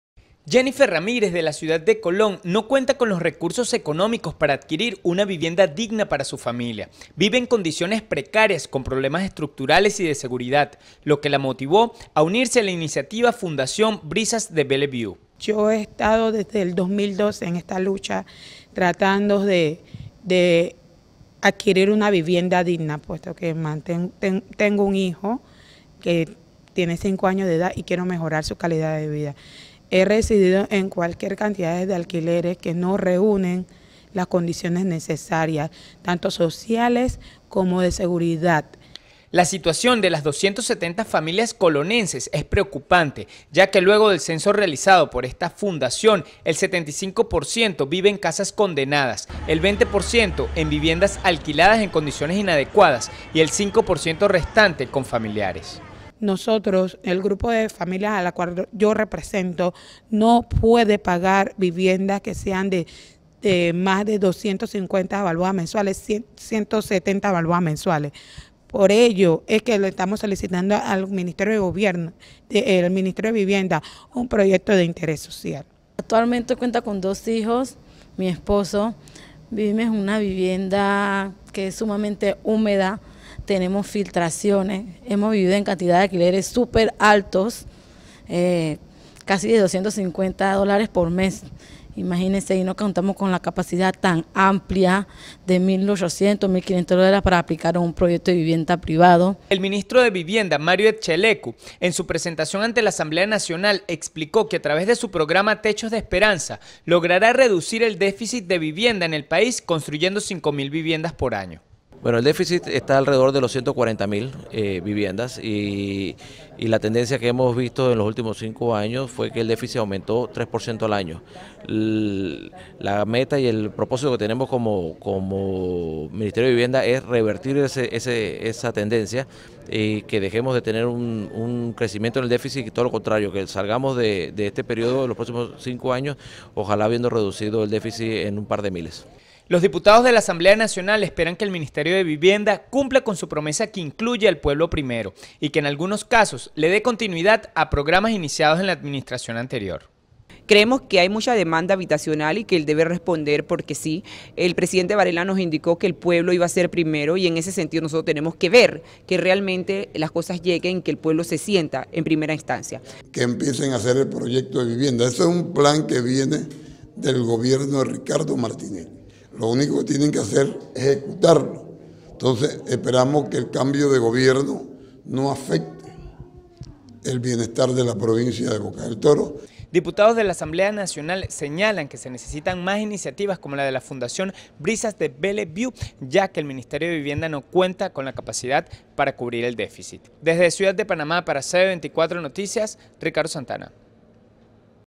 Panamá posee un déficit de 140 mil viviendas
En Panamá  existe un déficit de 140 mil viviendas con el aumento del 3% anual,  donde las condiciones precarias,  problemas estructurales,  escasez de sanidad básica e inseguridad  son el día a día de muchos ciudadanos.